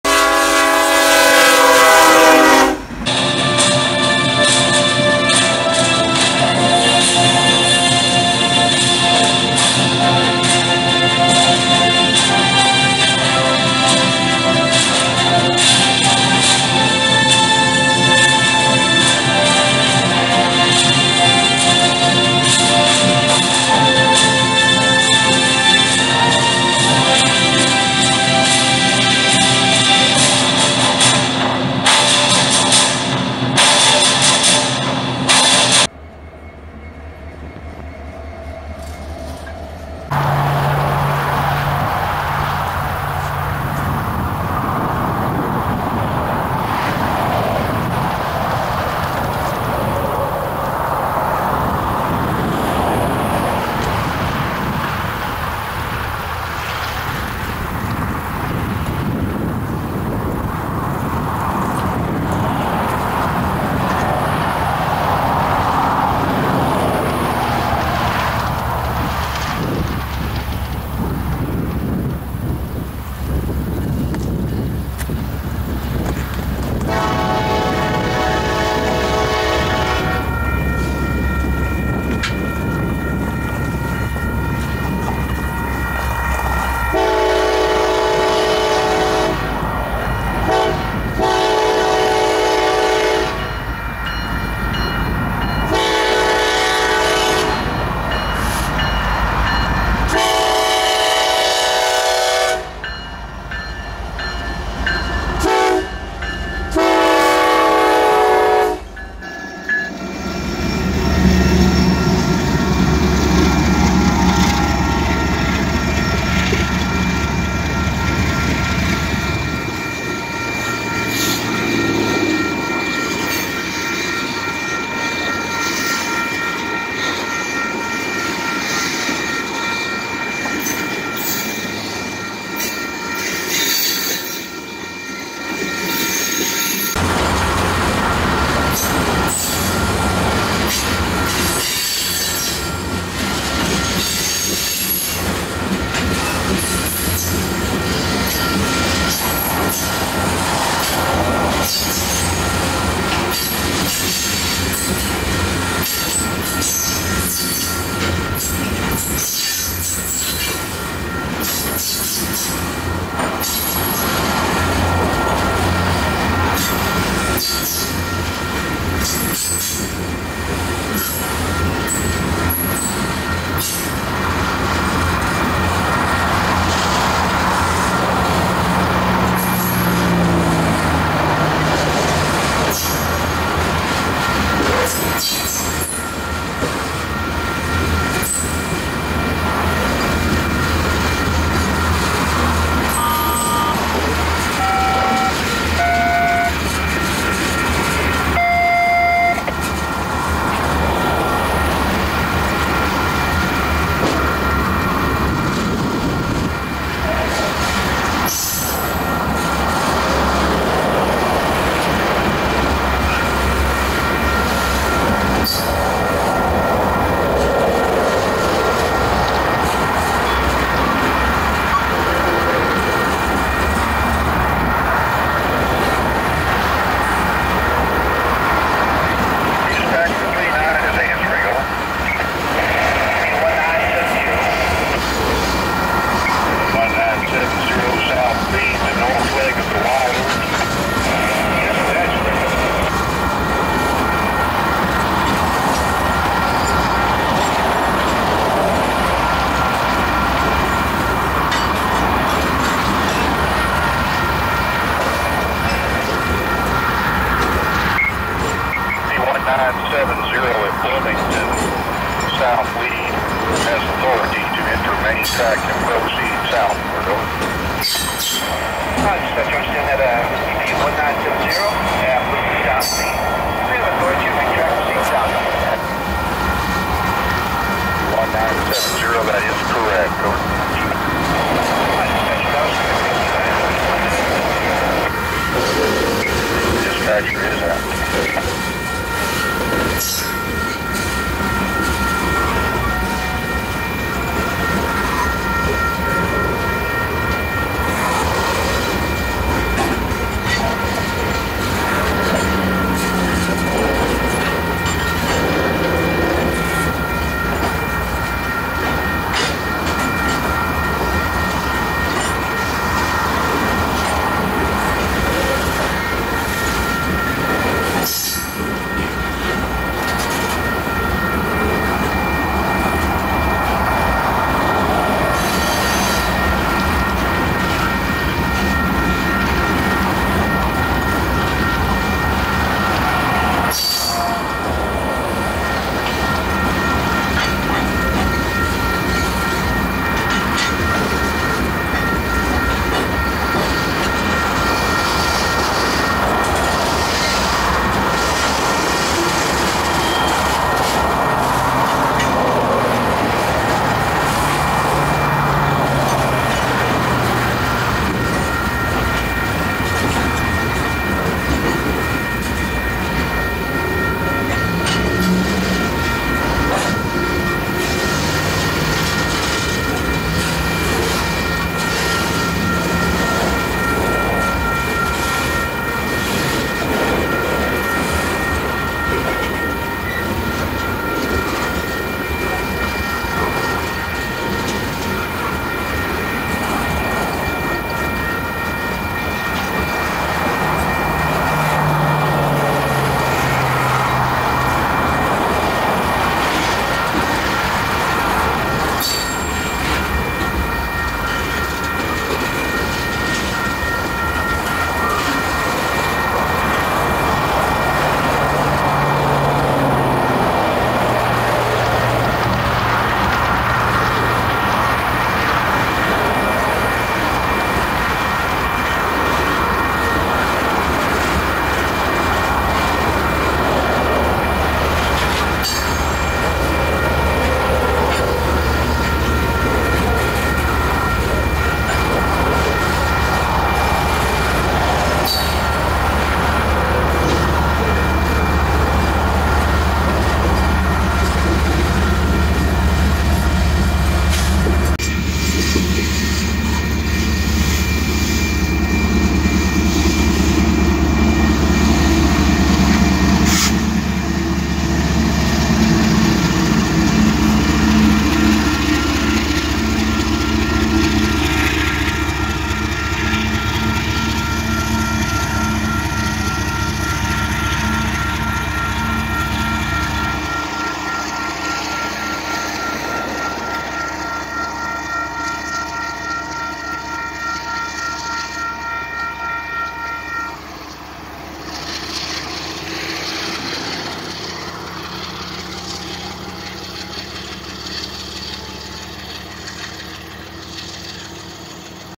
KCS #4113 Leads QHOQR-29
The QHOQR-29 passing the Henderson Road crossing near Placedo, Texas on the UPRR Cuero Subdivision.